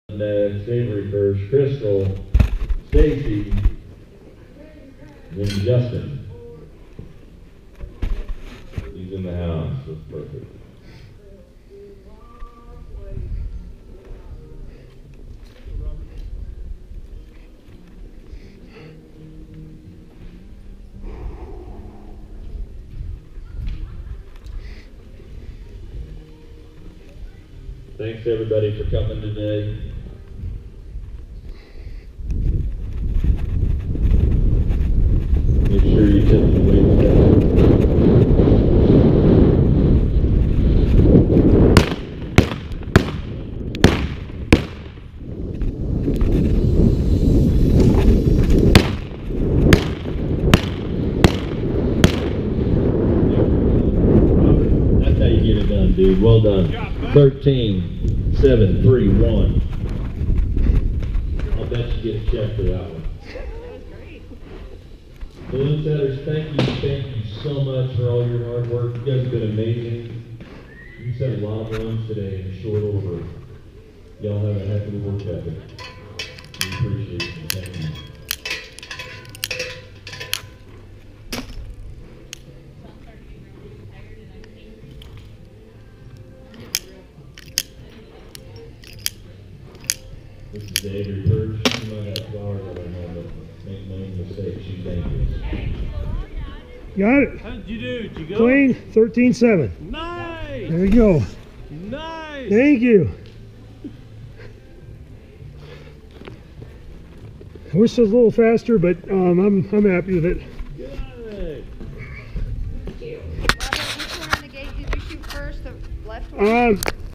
USMS Colorado Outlaws Mounted Shooting Event, S Lazy Ranch, Loveland, CO Stage 4
Here Renzi and I are running Stage 4 with a time of 13.731 seconds for 9th place overall